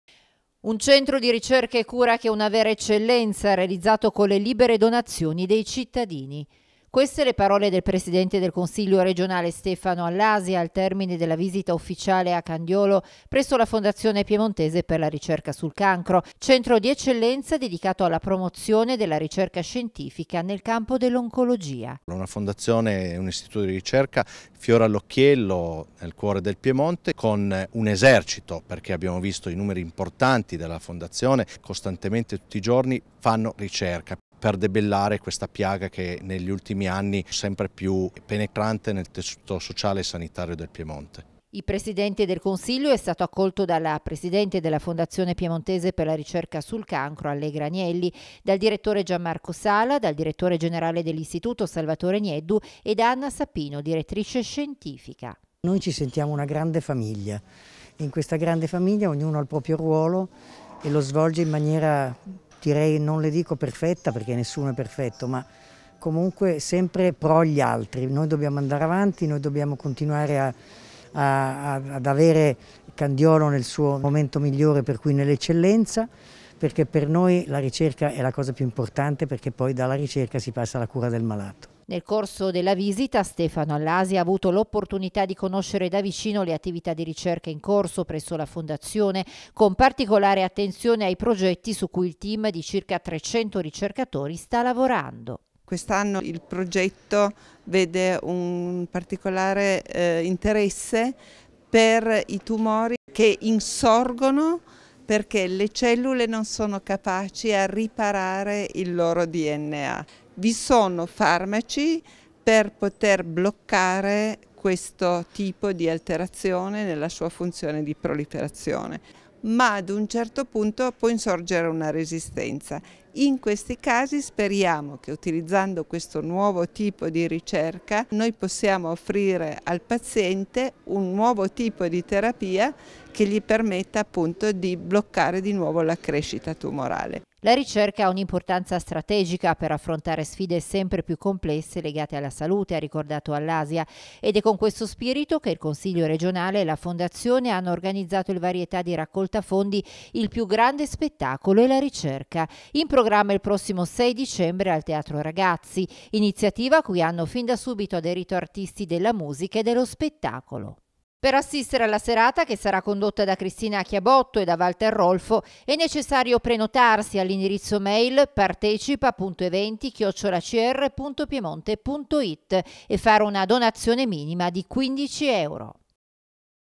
Allasia in visita alla Fondazione Piemontese per la Ricerca sul Cancro
“Una visita che mi ha permesso di vedere quanto in questi anni i semplici cittadini hanno contribuito a realizzare con le proprie libere donazioni: un centro di ricerca e cura che è una vera eccellenza”. Queste le parole del presidente del Consiglio regionale Stefano Allasia al termine della visita a Candiolo, alla Fondazione Piemontese per la Ricerca sul Cancro (FPRC), centro di eccellenza dedicato alla promozione della ricerca scientifica nel campo dell'oncologia